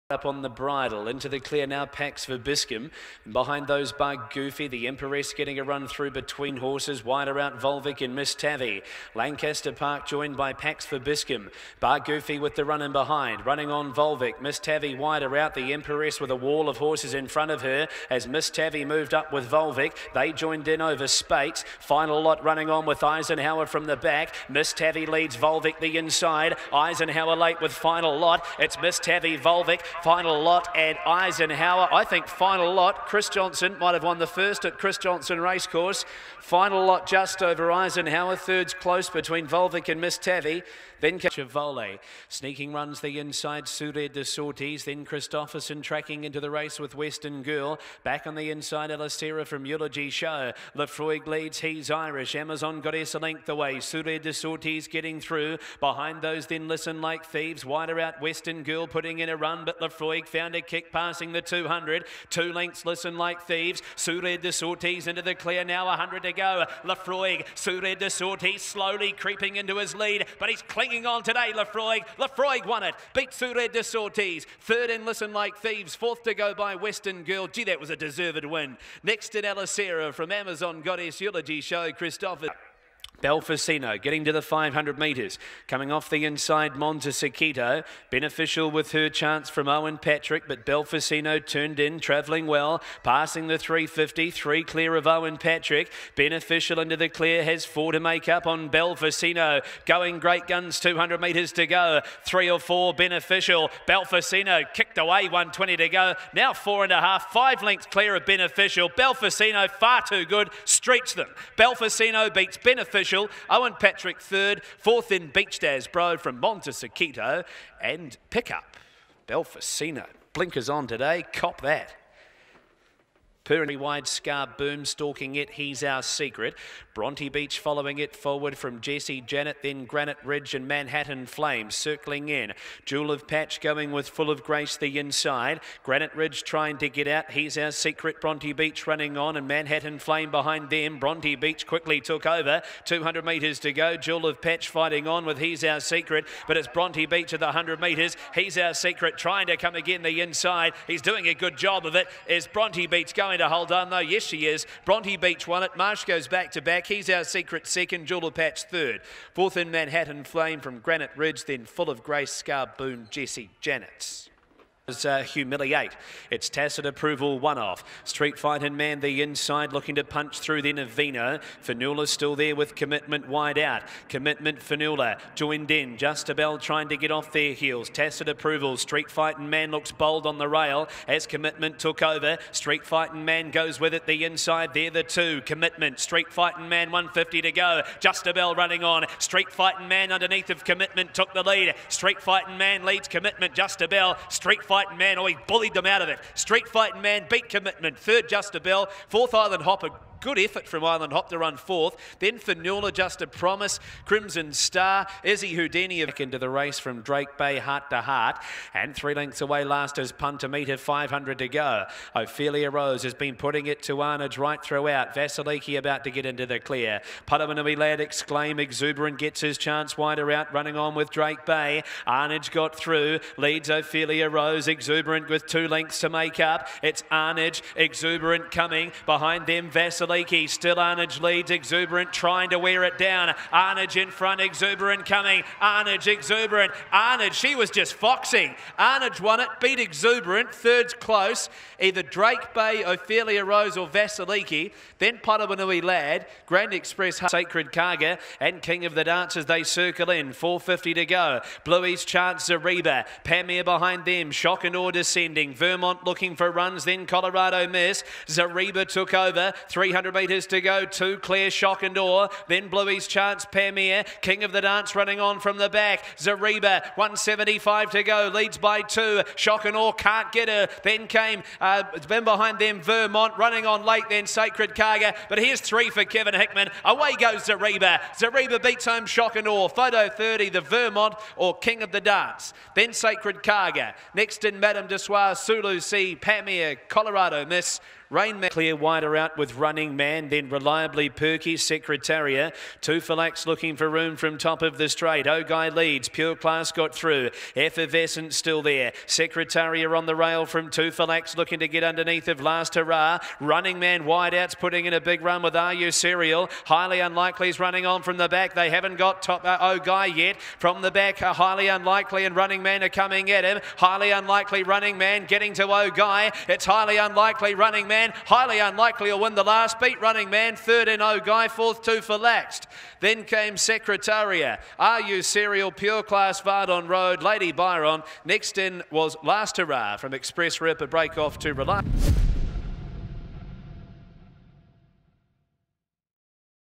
Final 400s Riccarton Park 21 January
Watch the final 400s of the 9 races from the Chris Johnson Raceday at Riccarton park - 21 January.